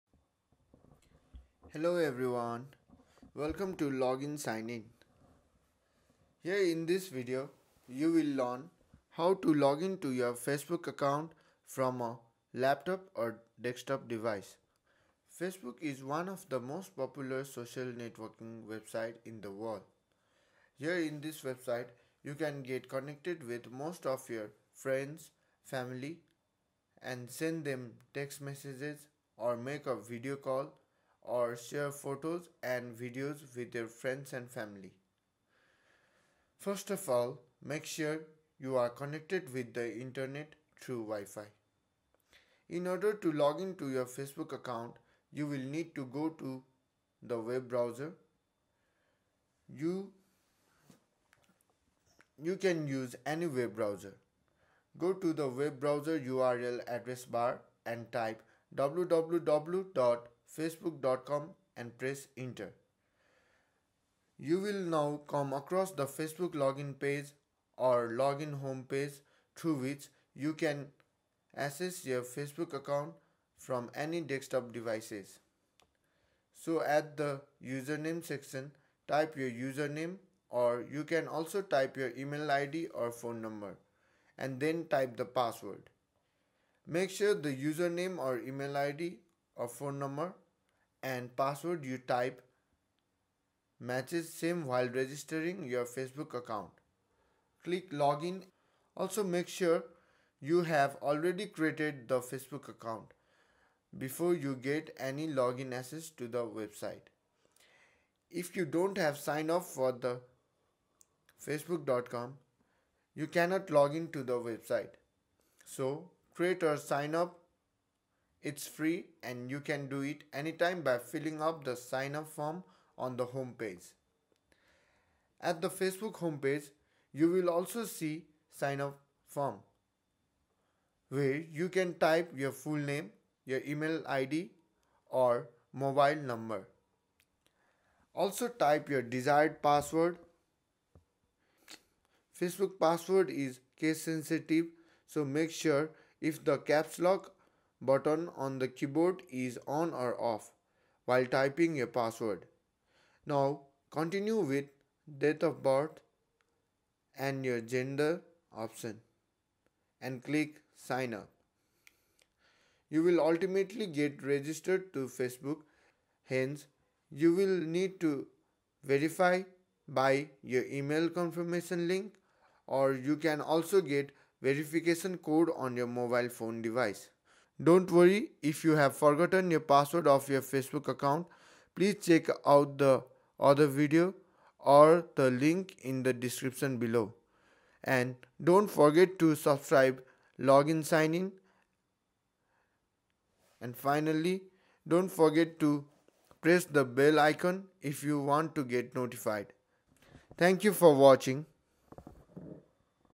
Login to Facebook.com | Facebook Login Sign In Steps on 2018 (Tutorial)
Welcome to facebook.com login help page. Here in this video you will learn about how to login to your facebook account. This is the latest and 100% working Facebook login steps of 2018.

We will bring you more login tutorials on this Channel with latest updates from popular websites.